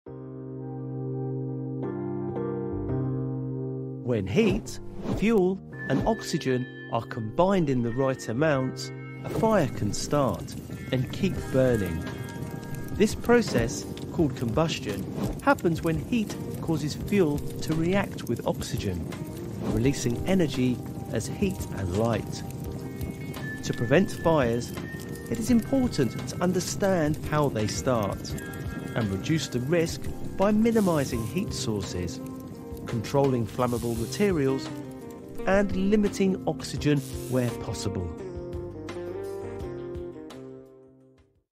The Fire Triangle and Fire Safety
This video is included as part of our full e-learning course, Fire Safety at Work and is provided here to give you helpful information on the topic. You can view the full e-learning course